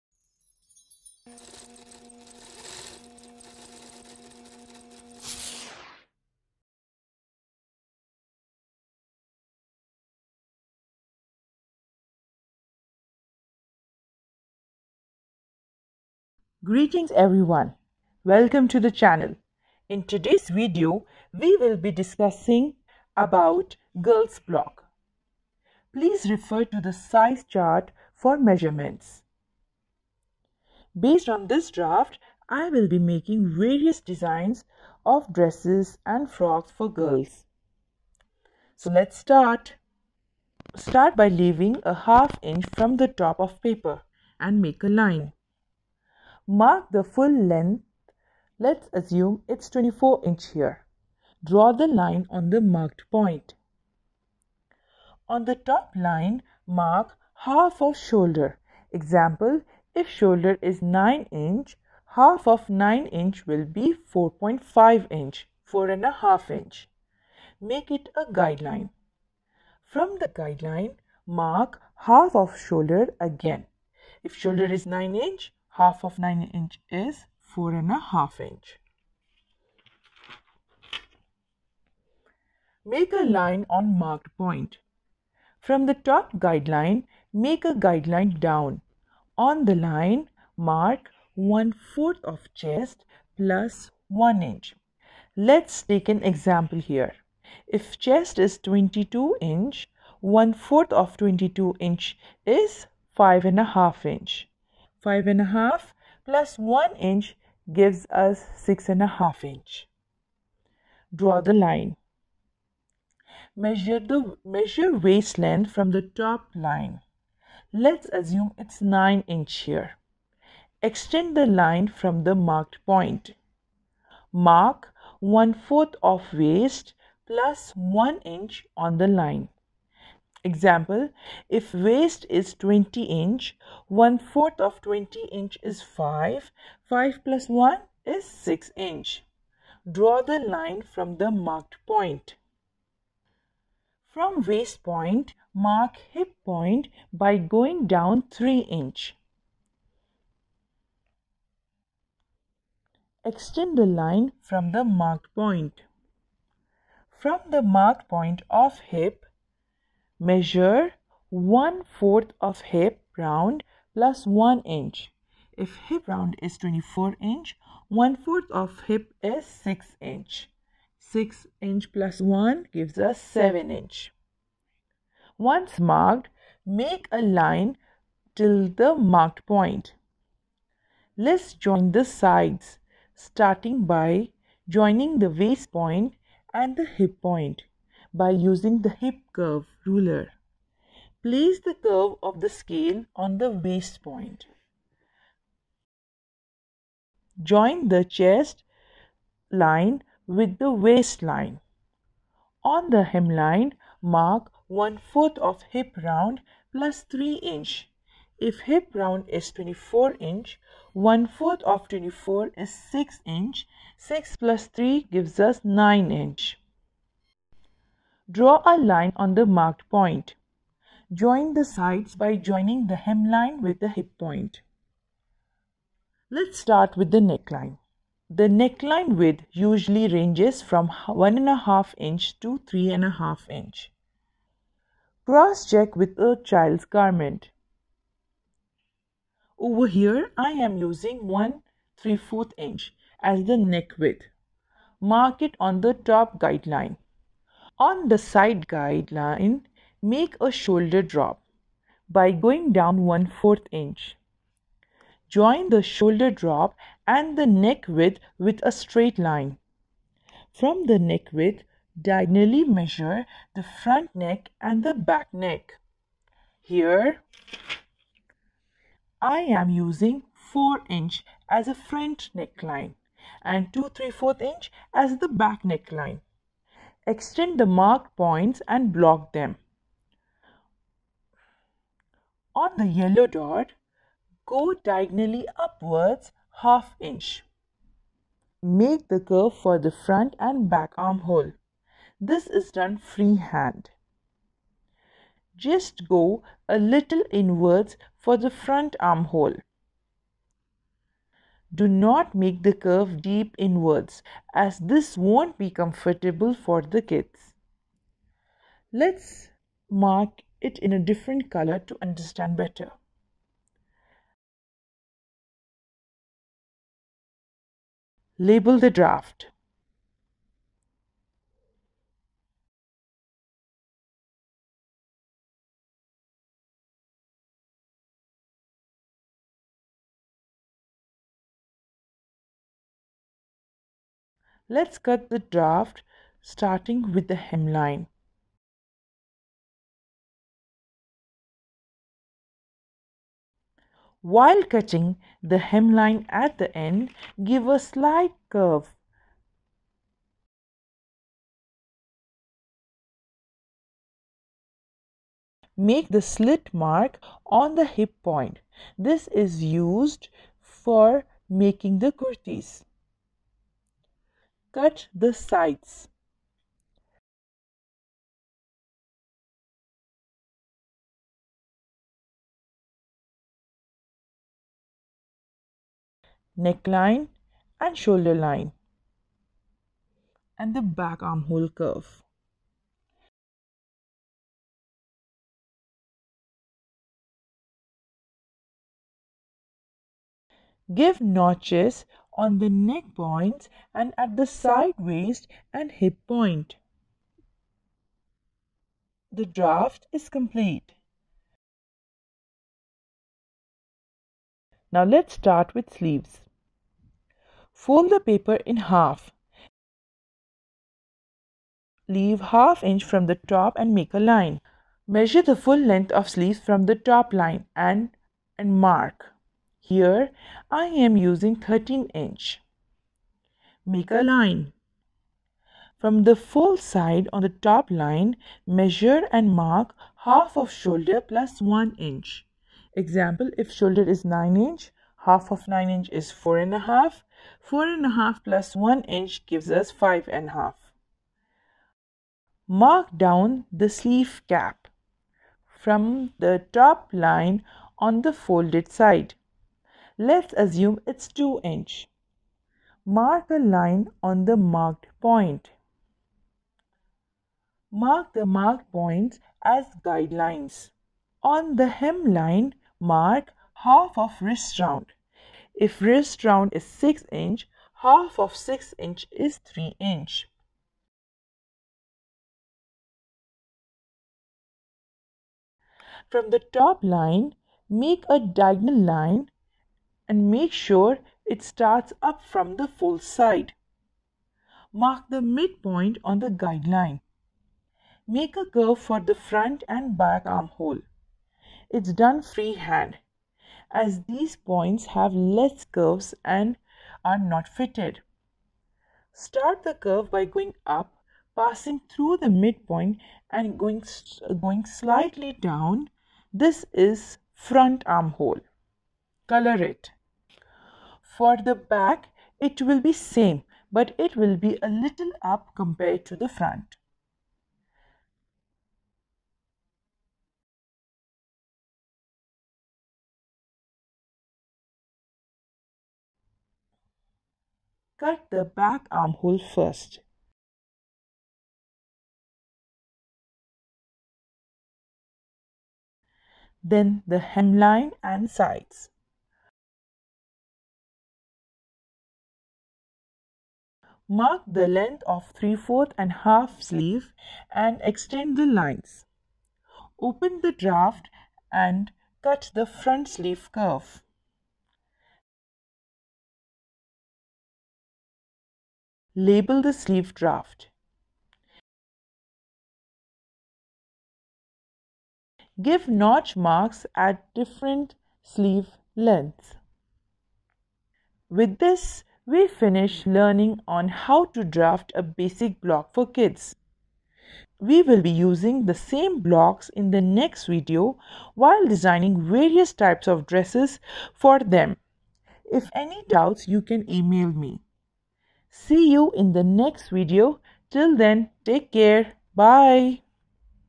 how to make a draft for baby / girls to make frock kameez and kurti | on paper
in this video learnhow to make a draft for baby / girls to make frock kameez and kurti | on paper for beginners will help you make your own draft on paper like a professional. as a beginners this video follows easy step by step guide how to make kurti draft for girls . as beginner it will be easy for you to follow the simple steps of how to make kurti kameez or frock.

😊"let us know if you are not able to understand or have doubts we are here to help"👍